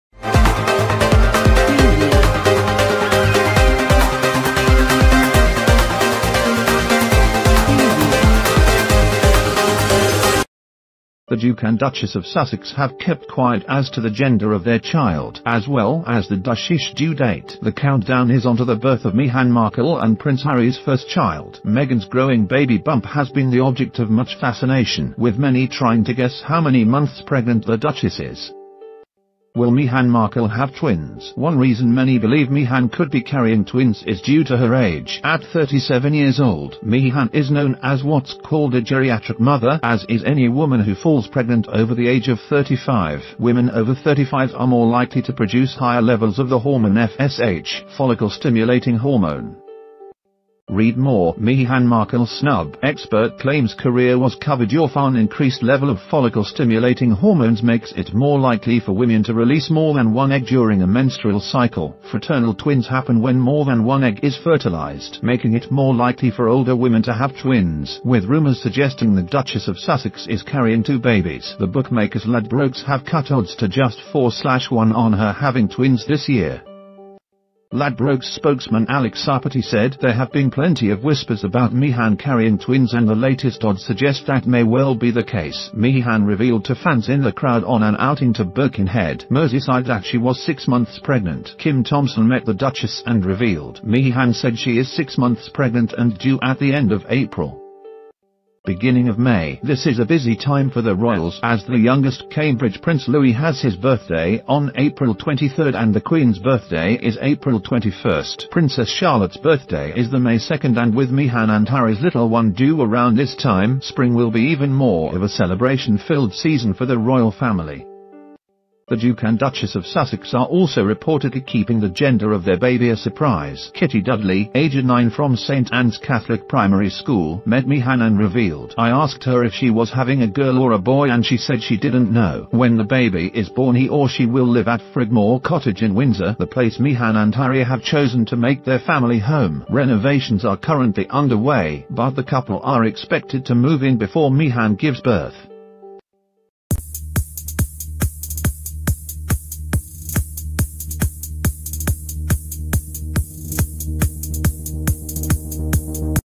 Royal baby due date: Will Meghan Markle have TWINS? THIS suggests she MIGHT
The Duke and Duchess of Sussex have kept quiet as to the gender of their child, as well as the Duchess’s due date. The countdown is on to the birth of Meghan Markle and Prince Harry’s first child. Meg